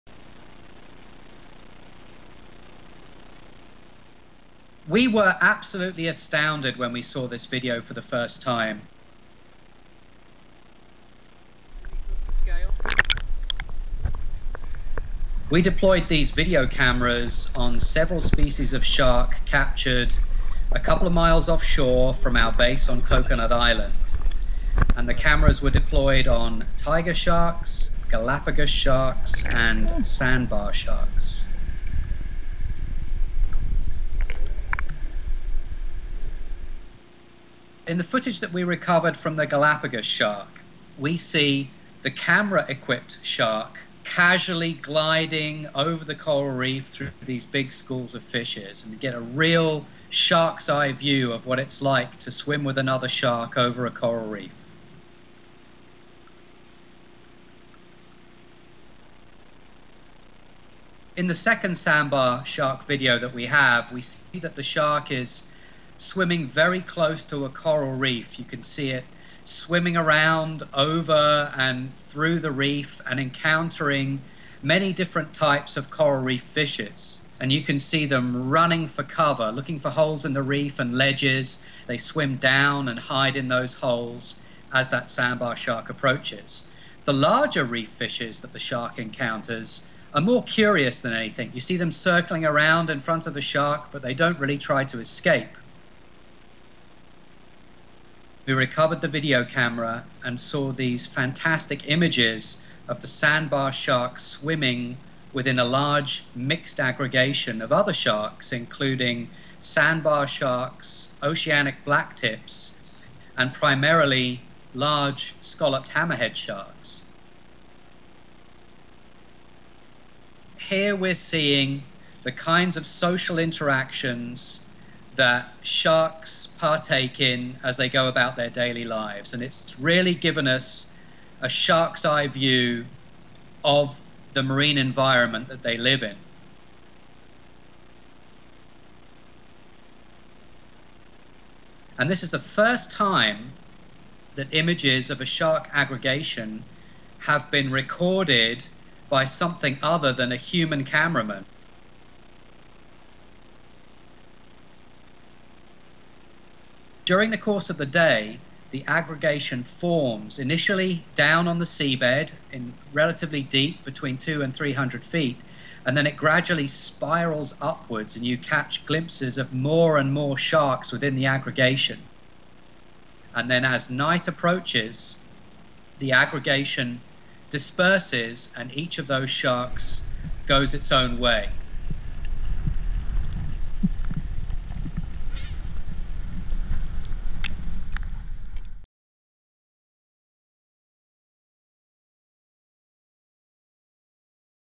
Social Sharks: "First" Video Evidence | National Geographic
Feb. 27, 2014—Some shark species have long been known to school (or form groups with their own kind), but now, for the first time, researchers near Hawaii have documented schooling of several different shark species. The researchers affixed small cameras to shark fins to see this behavior without any human presence.

PRODUCER: Jeff Hertrick
EDITOR: Nick Lunn